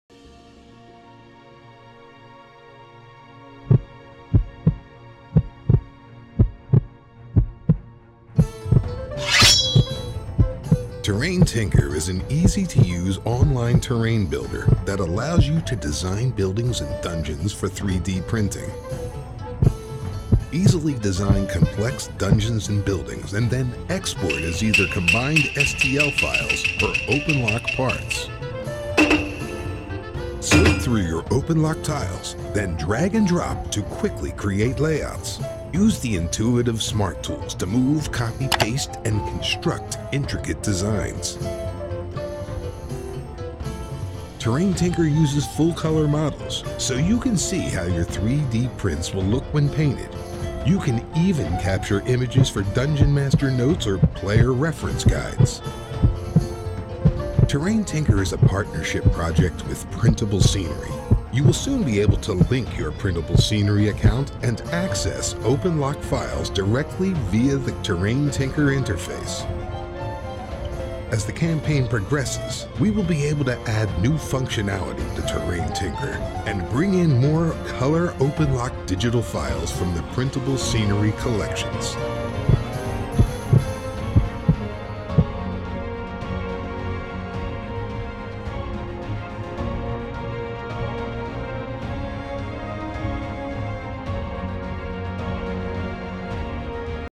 TERRAIN TINKER ONLINE DESIGN 3D PRINTABLE DUNGEONS BUILDINGS JOINT PRINTABLE SCENERY...2021 NZEALAND
TERRAIN TINKER ONLINE DESIGN 3D PRINTABLE DUNGEONS AND BUILDINGS JOINT VENTURE WITH PRINTABLE SCENERY...2021 NZEALAND

An online 3D builder to design your own 3D Printable Buildings and Dungeons using OpenLOCK tiles. Joint venture with Printable Scenery
Terrain Tinker is an easy-to-use online terrain builder that allows you to design buildings and dungeons for 3D printing. Terrain Tinker has been developed with Printable Scenery over the last year to create a practical terrain building application that uses all of the Printable Scenery OpenLOCK collection. When you're finished designing, you can export your creation as a print-ready combined model or separate OpenLOCK parts.
Terrain Tinker is a partnership project with Printable Scenery. You will soon be able to link to your Printable Scenery account and access your OpenLOCK files directly via the Terrain Tinker interface. Once the campaign closes we will send you a link so that you can link your Printable Scenery account to Terrain Tinker
The Beta is running smoothly, but we want to add more features and more high-detailed tiles.

We have a few things already planned and underway including;

Cutting & grouping for 3D printing.
Send your creation to licensed printers for printing and delivery to your door.
If things go really well we would love to add some crazy features such as:

 Unlocking all of the Printable Scenery's OpenLOCK tiles!
Scatter placement - the ability to place scatter terrain anywhere in the builder.
Import your own STL files into the builder.
Object collision detection.
Top-down isometric renders, for printing on paper.
A sharing platform to share your designs with others who have the same collections.
OpenLOCK Clips on the digital face for creating connectable larger building parts.
3D full color output for games like Tabletop Simulator.
Click and drag dynamic roof tool to easily design complicated custom roofs.
Automatic OpenLOCK port alignment - pick any style of object and we will handle getting the right OpenLOCK ports layout.
Fully combined single shell model export.
2D room tool for quickly designing rooms and letting Terrain Tinker do the calculations of which OpenLOCK tiles to use.
Collaboration with other OpenLOCK and 3d scenery designers to offer the largest range we can.
Dynamic floor level / roof heights
Store a list of what OpenLOCK parts you have already printed so you print only the extra parts you need
 As the campaign progresses, we will be able to add new functionality to Terrain Tinker and bring in more color OpenLOCK Digital files from the Printable Scenery collection.
If you would like any of the add-ons add the listed amount to your pledge.
Once the campaign closes you will be sent a link to the pledge manager to confirm the add-ons that you picked.

If you already own Clorehaven or would prefer to just get the other collections you can back at the base pledge level and add-on the extra collections you would like